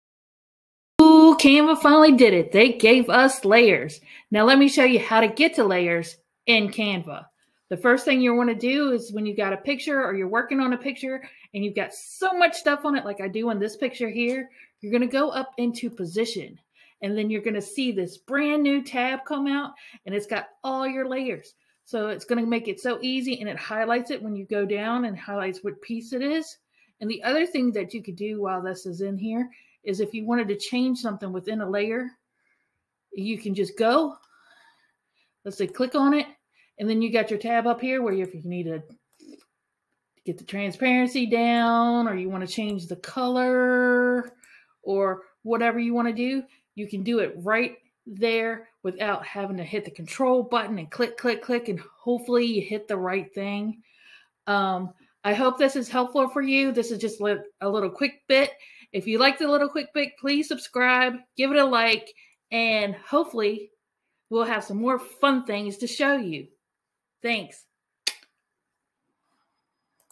How to Find the New Layers Tab in Canva
Did you know Canva now has a layers feature to make your designing process even easier? This tutorial will show you where to find the layers tab and how easy it is to use.

Canva basics for beginners

All videos are 100% made in Canva